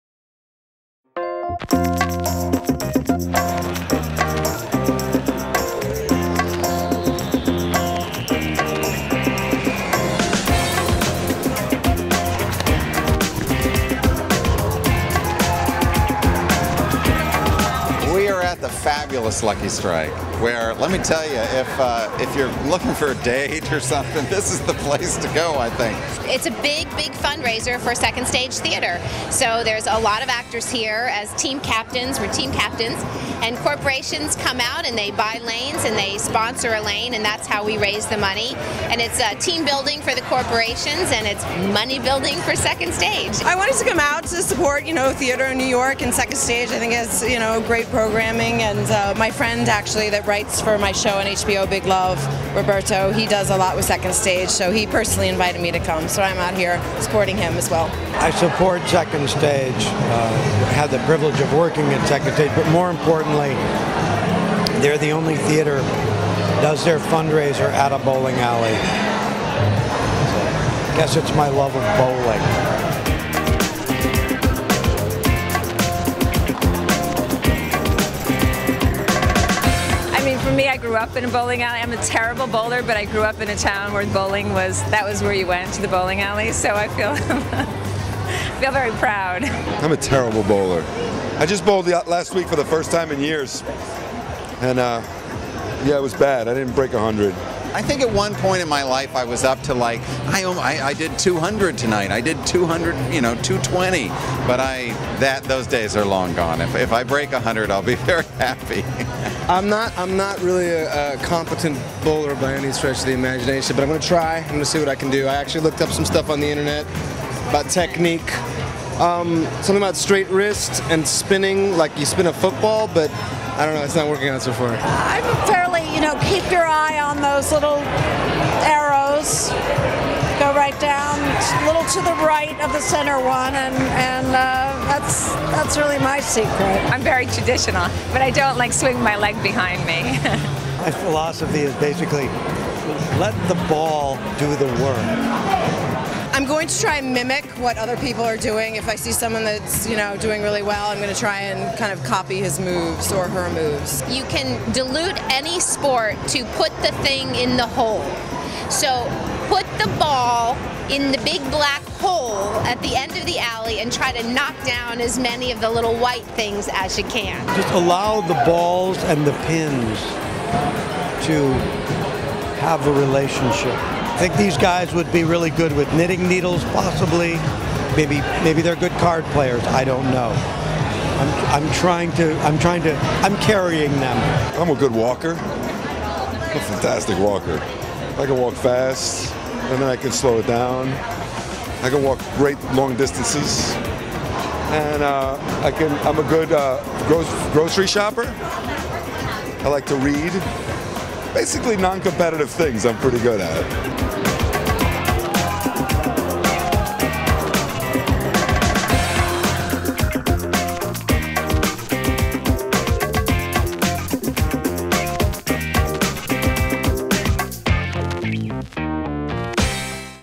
On the Scene: Second Stage Supporters Go Bowling!
Celebs come out to bowl for off-Broadway's Second Stage Theater.